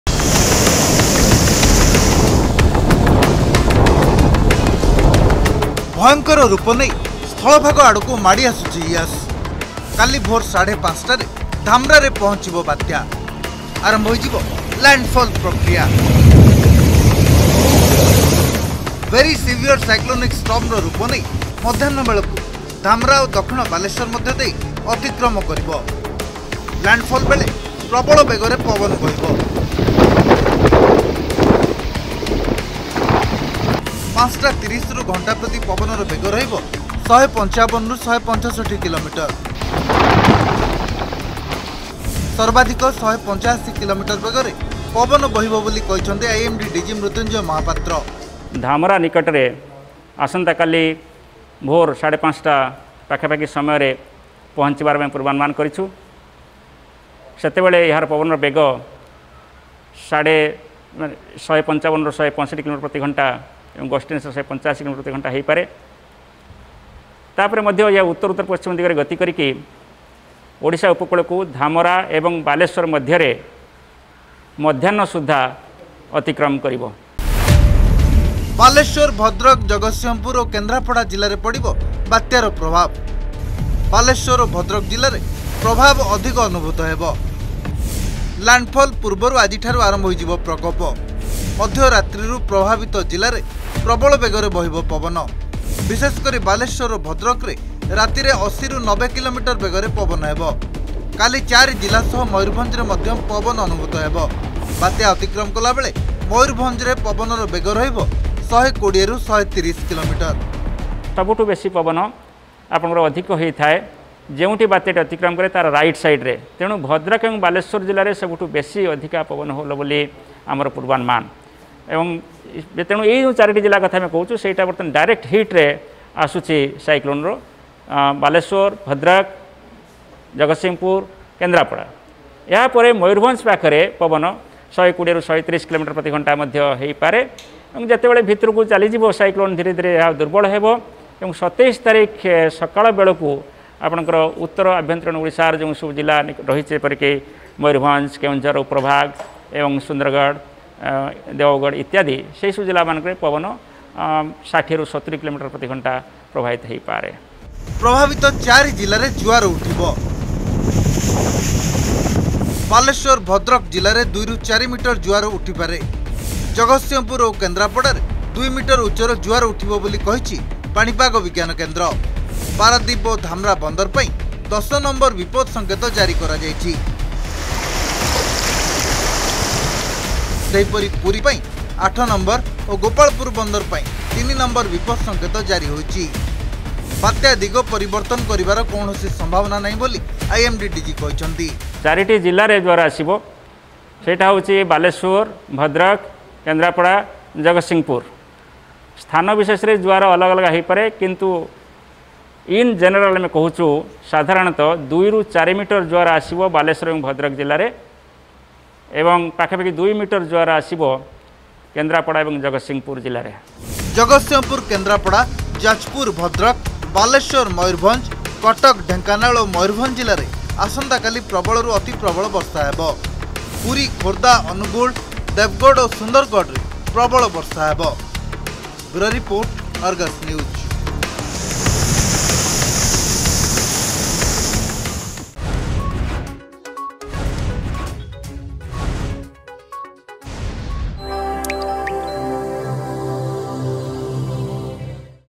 ପାଖଉଛି ବାତ୍ୟା ୟଶ୍ ।
ପାଖଉଛି ବାତ୍ୟା ୟଶ୍ । କାଲି ଭୋରରେ ପହଞ୍ଚିବ ଧାମରା । ମଧ୍ୟାହ୍ନରେ ଧାମରା-ବାଲେଶ୍ବର ଦେଇ କରିବ ଅତିକ୍ରମ । ବାଲେଶ୍ବର, ଭଦ୍ରକ ସହ ଜଗତସିଂହପୁର ଓ କେନ୍ଦ୍ରାପଡ଼ା ଜିଲ୍ଲାରେ ପଡିବ ବାତ୍ୟାର ପ୍ରଭାବ । ସର୍ବାଧିକ ୧୮୫ କିଲୋମିଟର ବେଗରେ ପବନ ବହିବ । ୨ରୁ ୪ ମିଟର ଉଚ୍ଚର ଜୁଆର ଉଠିବ । ଏହା ସହ ବିଭିନ୍ନ ଜିଲ୍ଲାରେ ପ୍ରବଳରୁ ଅତି ପ୍ରବଳ ବର୍ଷା ହେବ